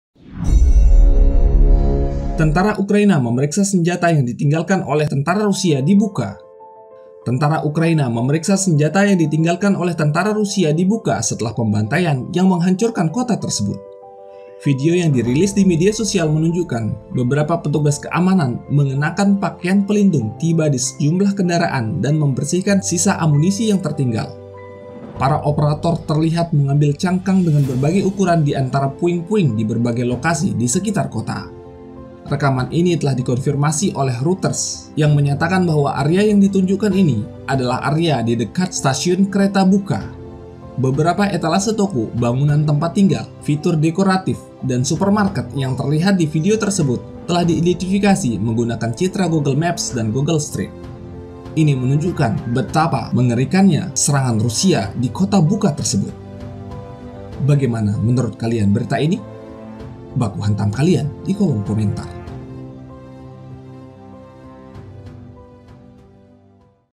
Tentara Ukraina memeriksa senjata yang 'ditinggalkan oleh tentara Rusia' di Bucha
Tentara UKRAINa memeriksa senjata 'ditinggalkan oleh tentara Rusia' di Bucha, setelah pembantaian yang menghancurkan kota tersebut.

Para operator terlihat mengambil cangkang dengan berbagai ukuran dari antara puing-puing di berbagai lokasi di sekitar kota.

Rekaman ini telah dikonfirmasi oleh Reuters untuk menunjukkan area di dekat stasiun kereta Bucha. Beberapa etalase toko, bangunan tempat tinggal, fitur dekoratif, dan supermarket yang terlihat di video telah diidentifikasi menggunakan citra Google Maps dan Google Street View.

Selamat menyaksikan video Tentara Ukraina memeriksa senjata yang 'ditinggalkan oleh tentara Rusia' di Bucha ini. terimakasih